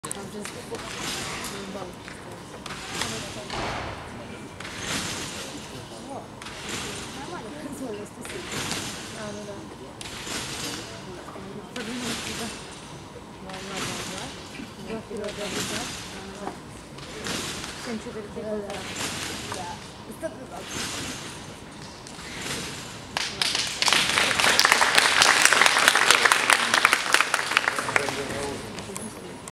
2. YAMASHITA Haruna JPN, ladies finals (HD)
Trampoline World Cup series finals, Astrakhan 2010. Videos are presented in starting order for the results please